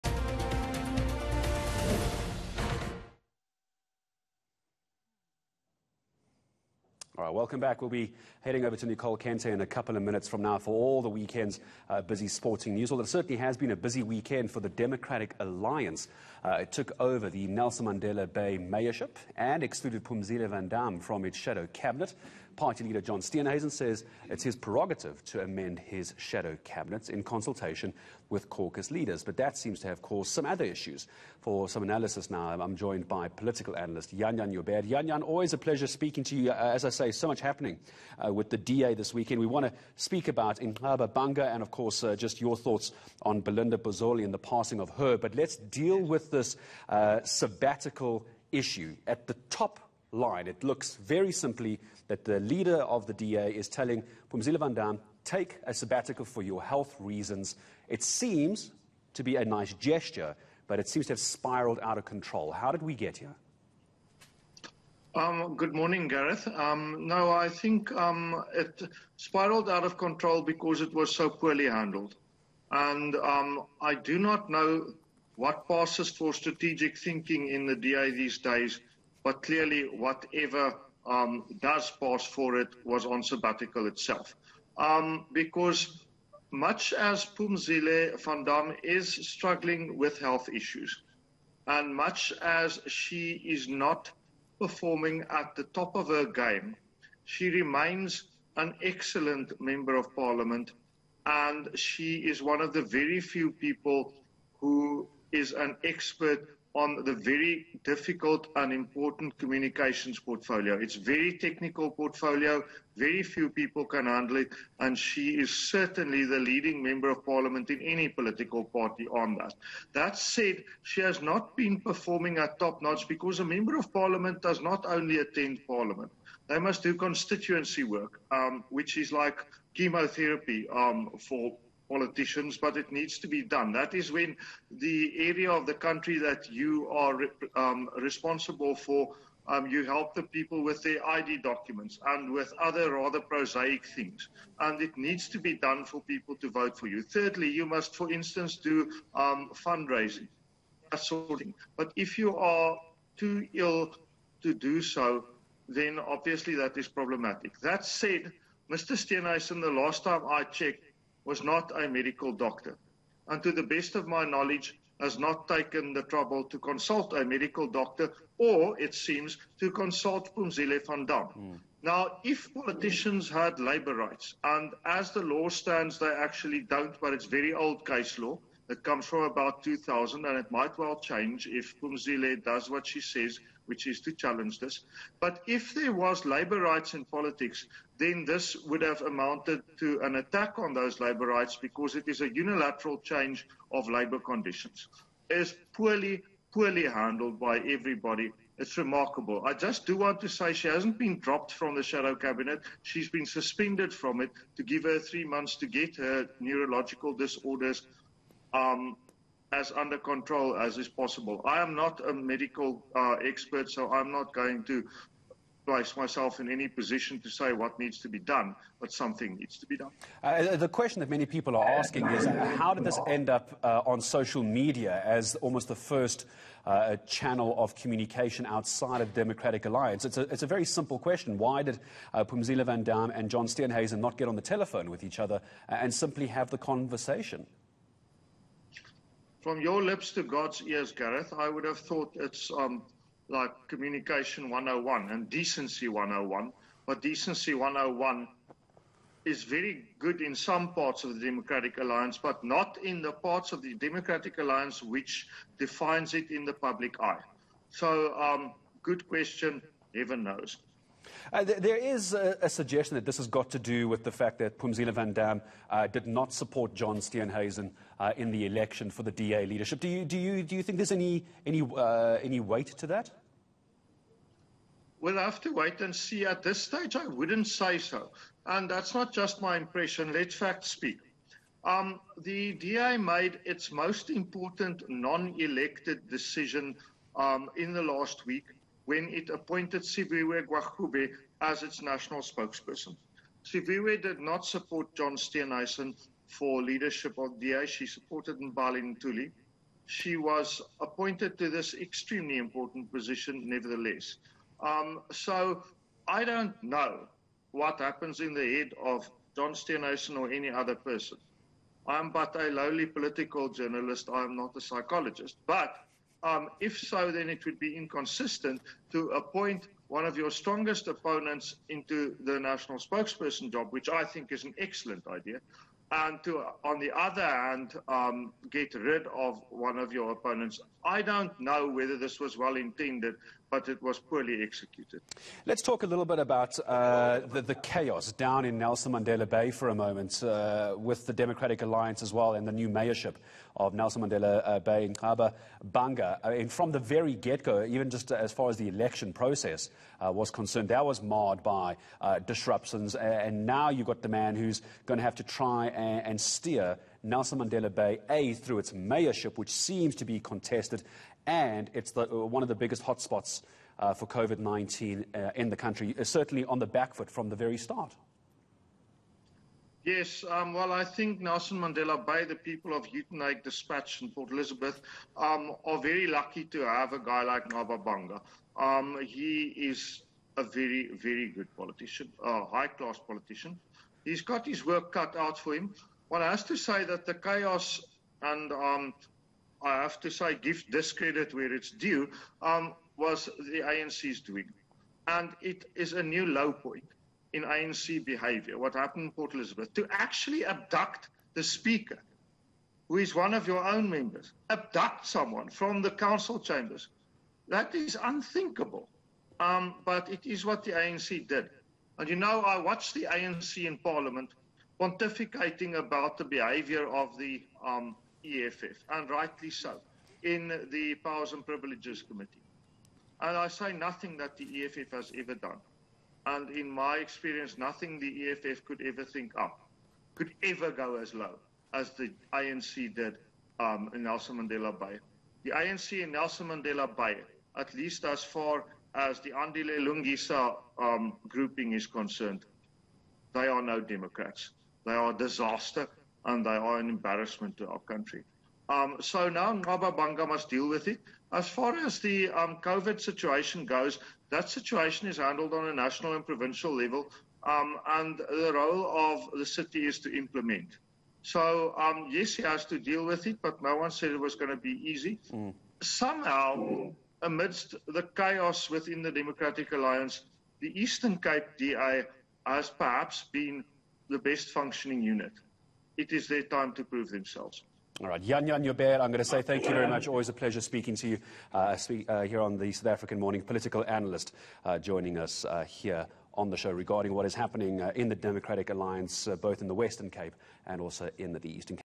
Discussion | A busy weekend for DA
The DA took over the Nelson Mandela Bay mayorship, and excluded Phumzile van Damme from its shadow cabinet.

Party leader John Steenhuisen says it's his prerogative to amend his shadow cabinet, in consultation with caucus leaders.

For some analysis, eNCA speaks to Jan-Jan Joubert.